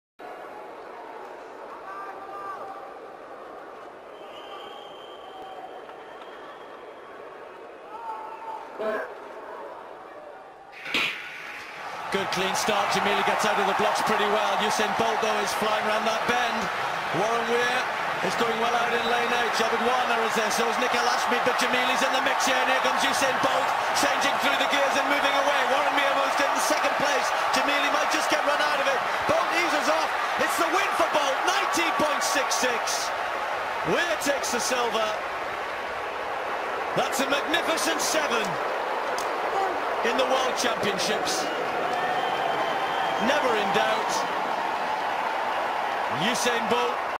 Usain Bolt Final 200M Moscow
Usain Bolt won the 100 metres title at the World Championships in Moscow minutes ago.